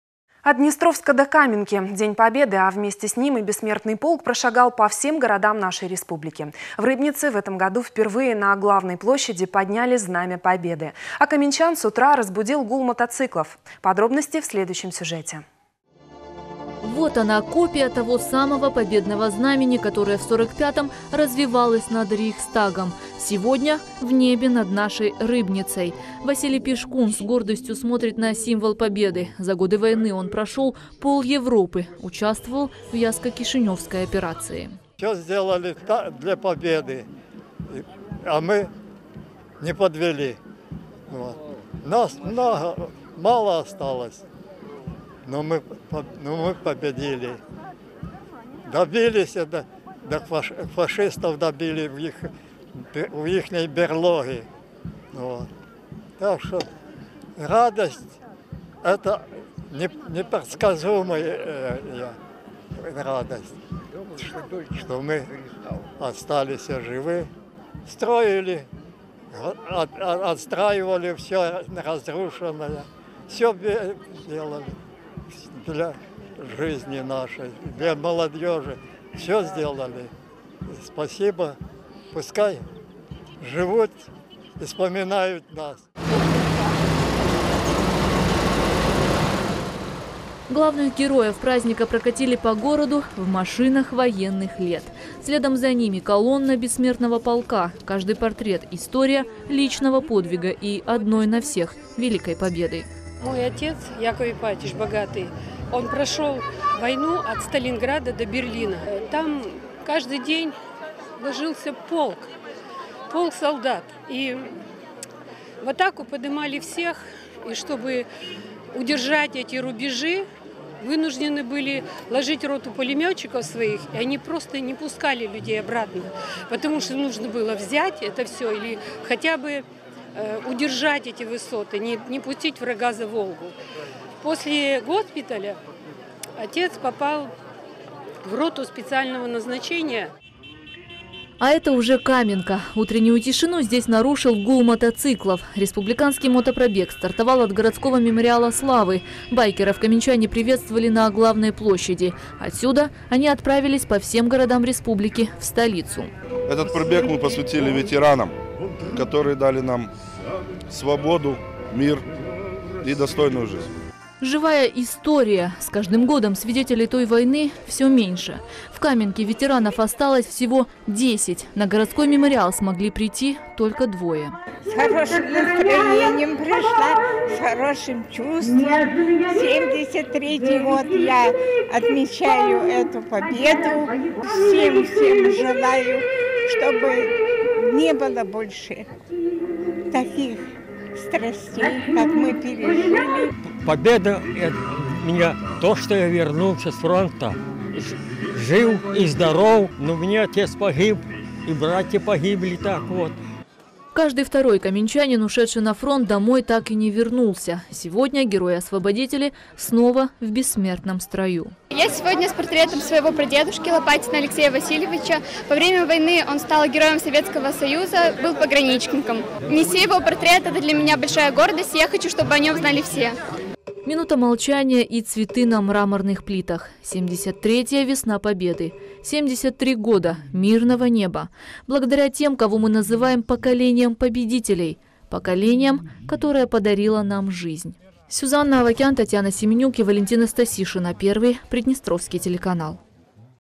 Весна Победы в Каменке и Рыбнице
От Днестровска до Каменки. День Победы, а вместе с ним и «Бессмертный полк» прошагал по всем городам нашей республики. В Рыбнице в этом году впервые на главной площади подняли Знамя Победы. А каменчан с утра разбудил гул мотоциклов.